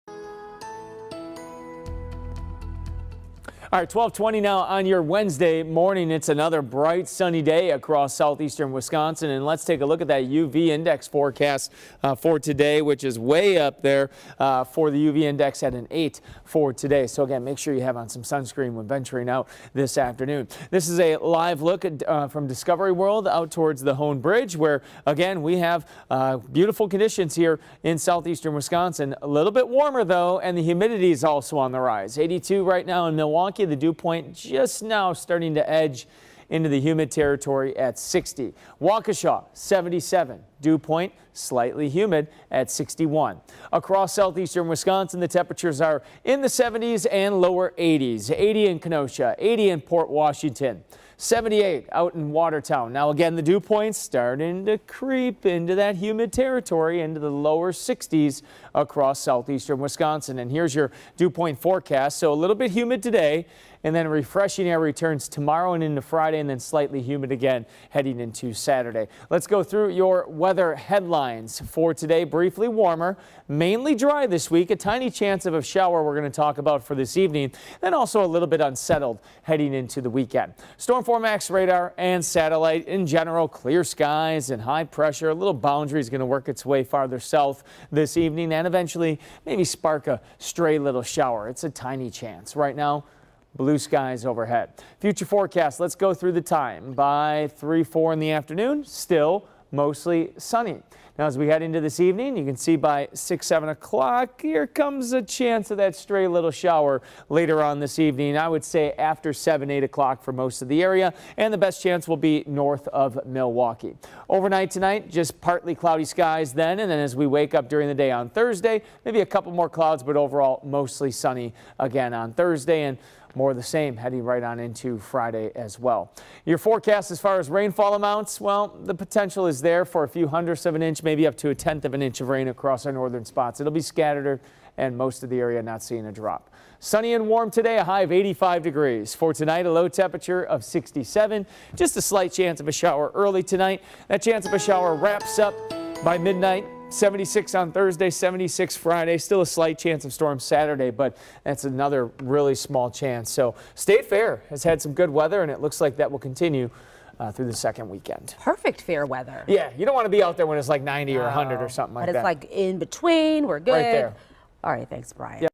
Showers possible for some Wednesday evening
Sunshine and dry weather is expected through the day, but a few showers may move into the area later this evening around sunset. Locations north of Milwaukee have the best chance of seeing a little bit of rain.